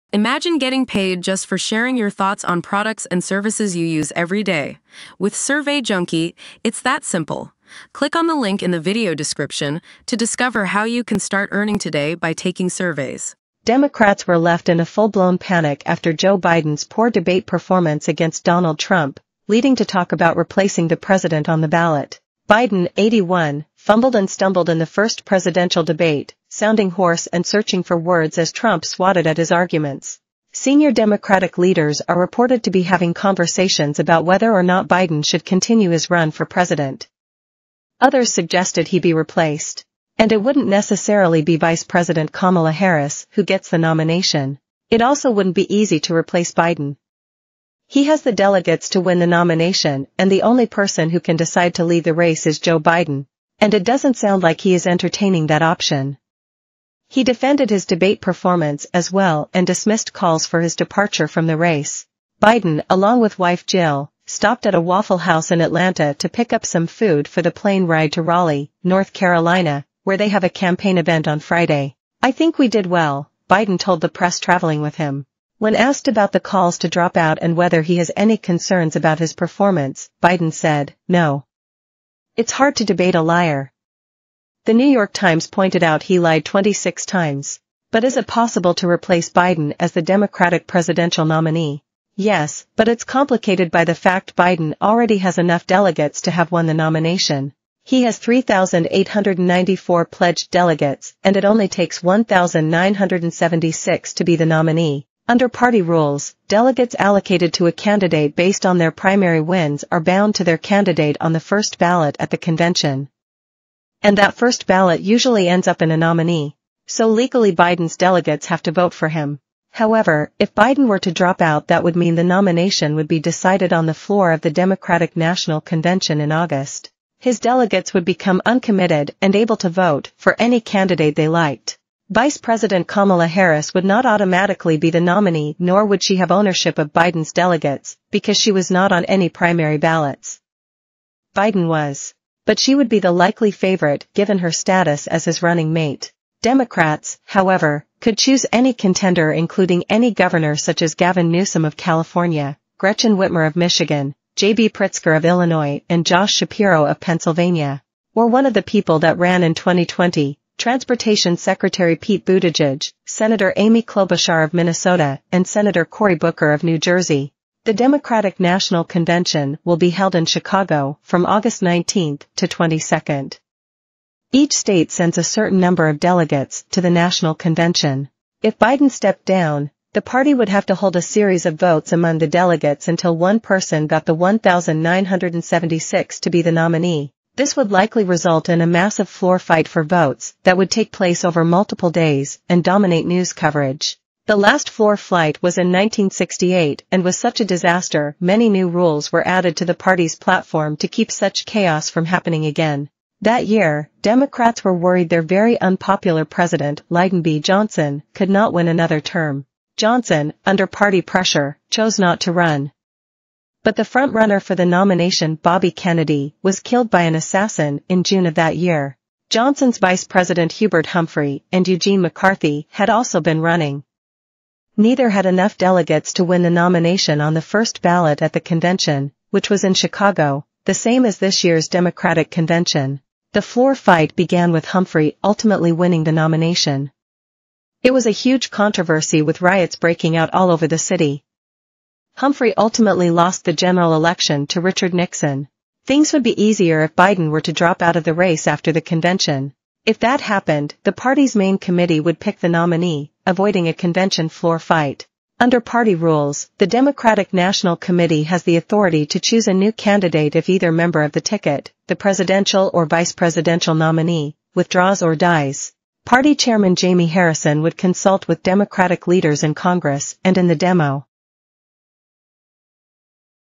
How Democrats CAN replace Biden after his disastrous debate
How Democrats CAN replace Biden after his disastrous debate performance